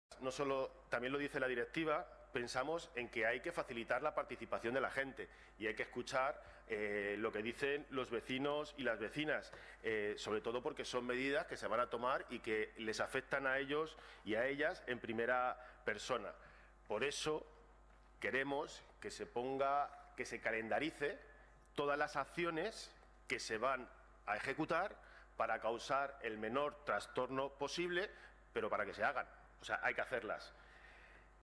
Intervención Moción para calendarizar las acciones de las ZBE Leganés
Hay que facilitar la participación  de la gente y hay que escuchar lo que dice los vecinos y vecinas , porque son medidas que les va a afectar en primera persona
Queremos que se calendaricen las acciones para que causen el menor trastorno posible.